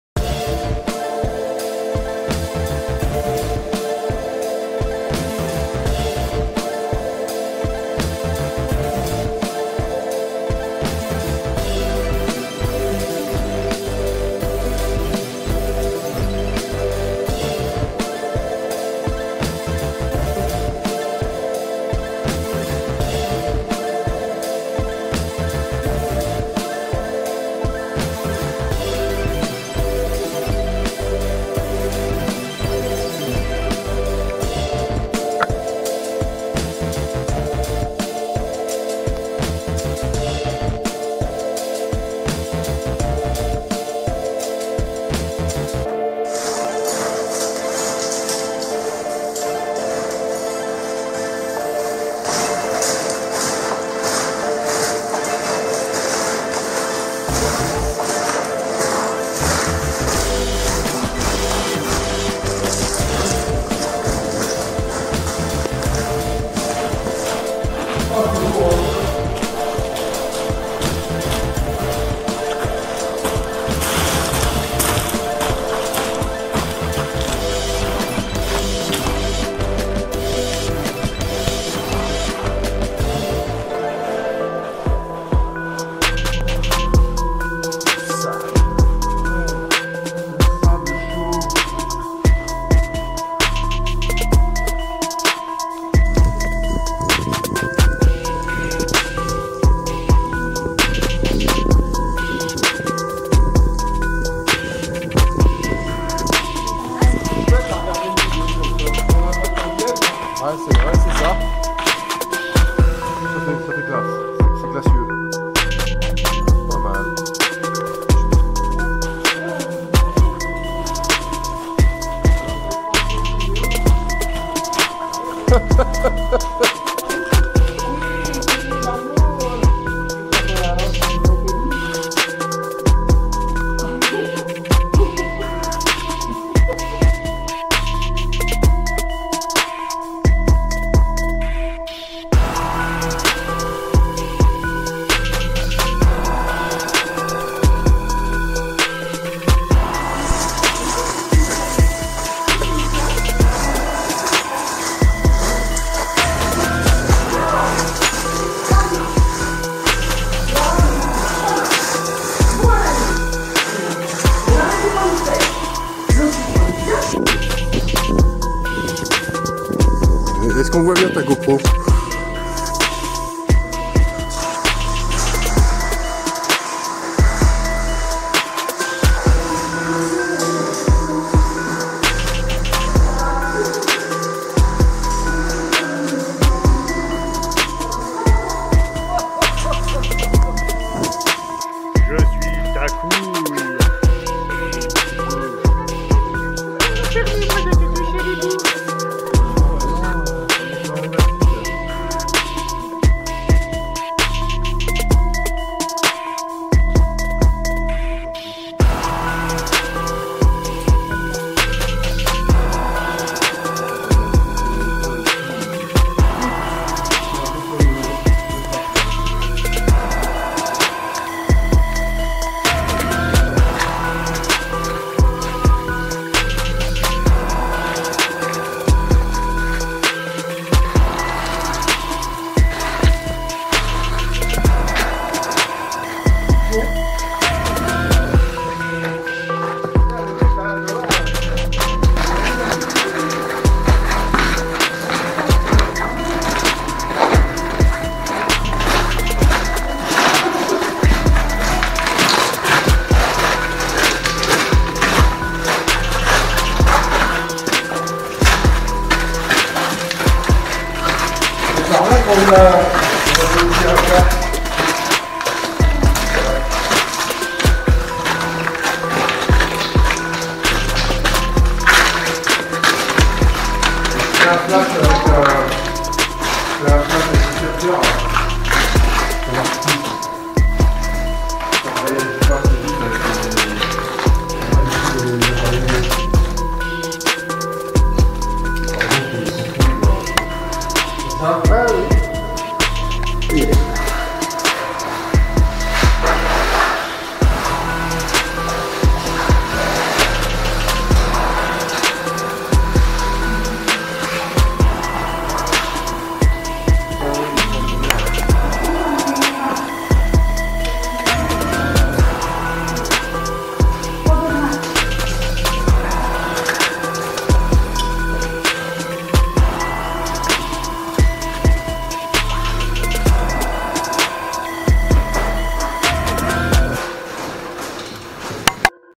Cotepatière Cocalière 03 2019 CRAD
Au matin du quatrième jour, le programme est à la détente. Roro Cassé, Domi rangée, le reste de l’équipe toujours motivée à s’engouffrer sous terre. Nous décidons d’aller faire une visite dans la Cotepatière et de suivre dans la Cocalière. La météo nous a abandonné pour aujourd’hui et c’est donc sous une petite pluie que nous allons affronter les risques de crues. Ce réseau est en effet connu pour ses crues subites et rapides qui se caractérisent par les divers débris collés au plafond du réseau. Même pas peur, nous maintenons le programme. Une fois équipés, nous croisons des autochtones qui cherchent encore l’aven de la Cocalière. Poulpi nous accompagne jusqu’à l’entrée du réseau – on dirait qu’il serait bien resté pour faire cette petite visite souterraine avec nous.